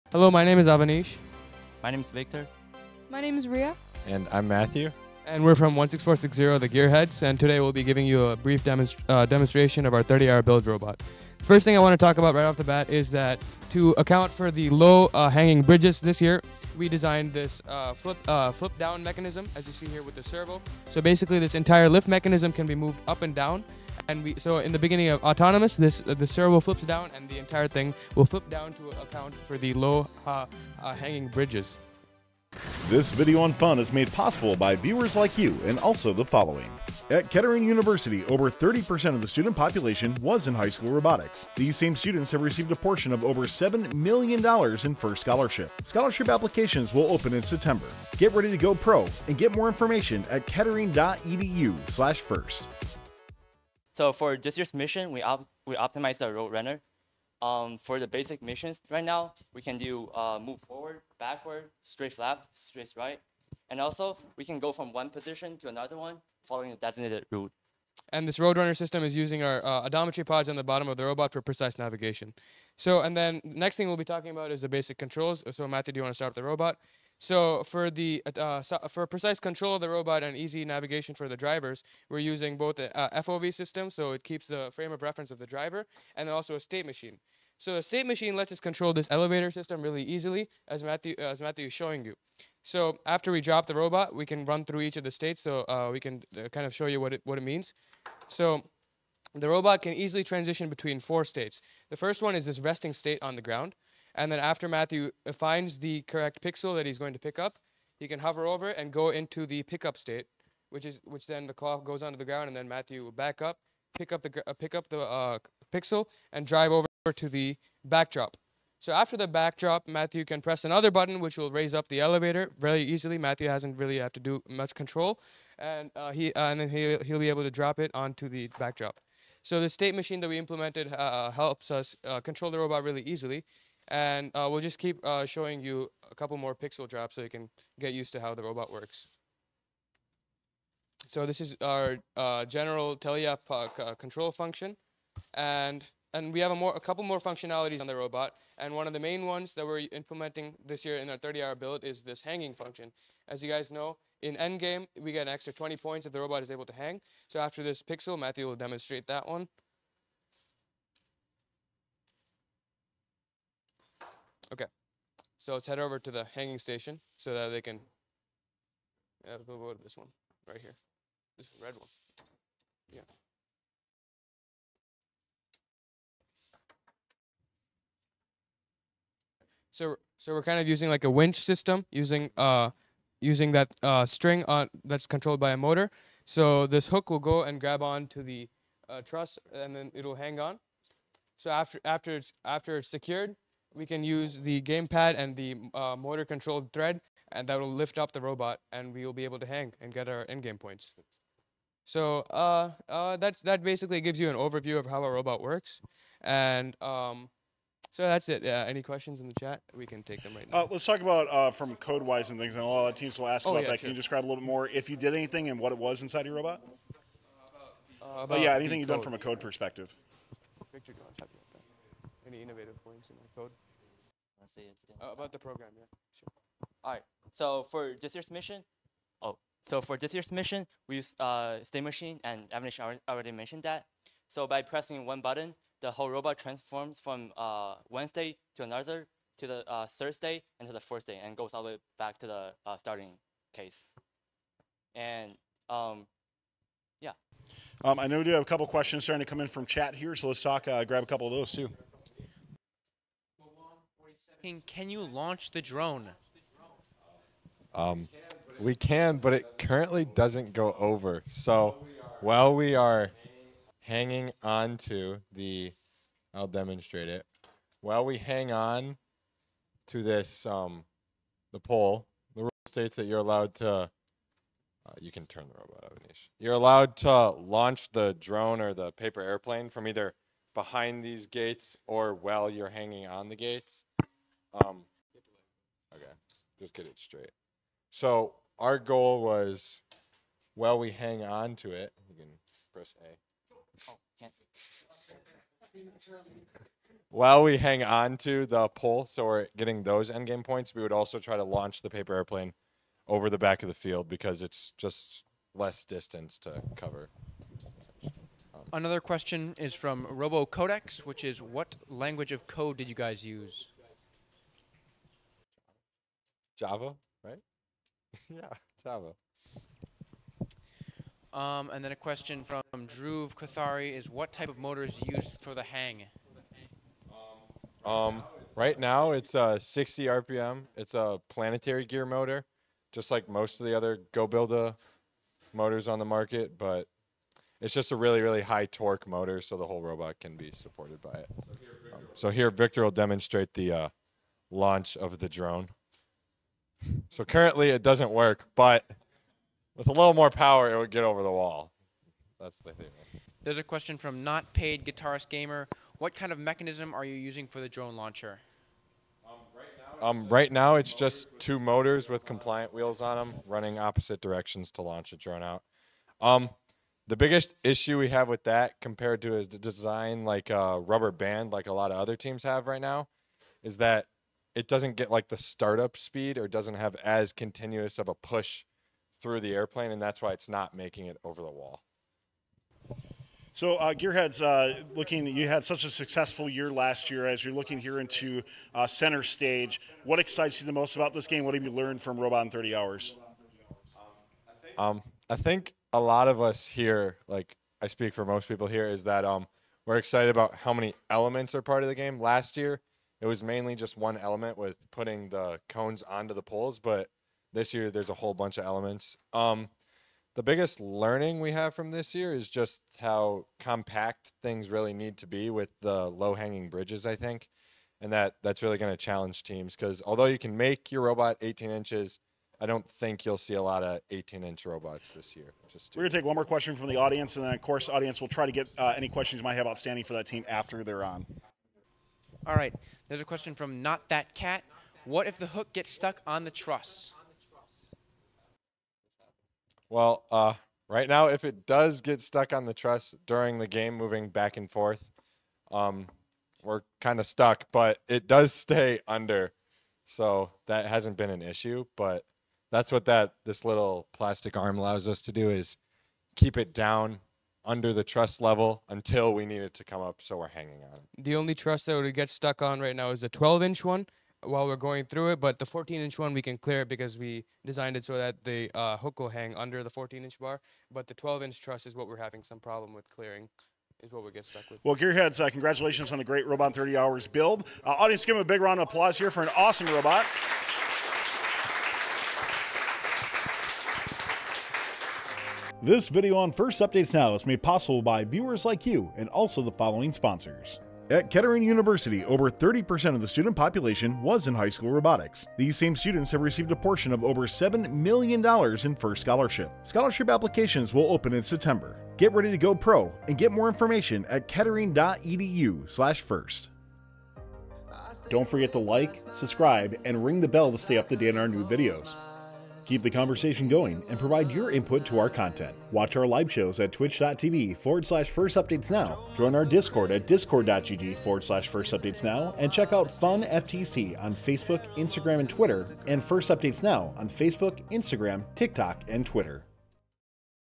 16460 GEarheads Robot Overview | Climber | Drone Launcher | Intake | Programming | Robot in 30 Hours
16460 GEarheads provides a demo and overview of their Robot in 30 Hours showing off their climber, drone launcher, intake, programming and more!